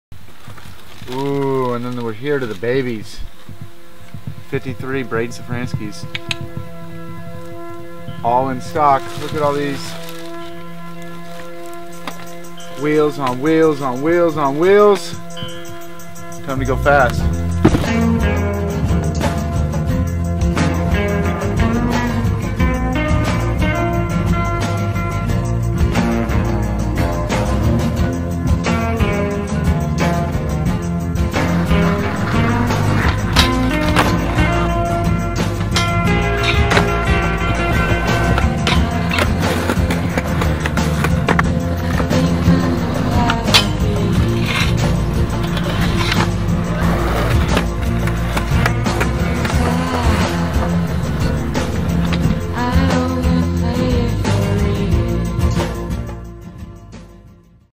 New Braydon Szafranski Ghetto Child Wheels available now
Check out Braydon Szafranski’s new pro wheel. or at fine skateshops.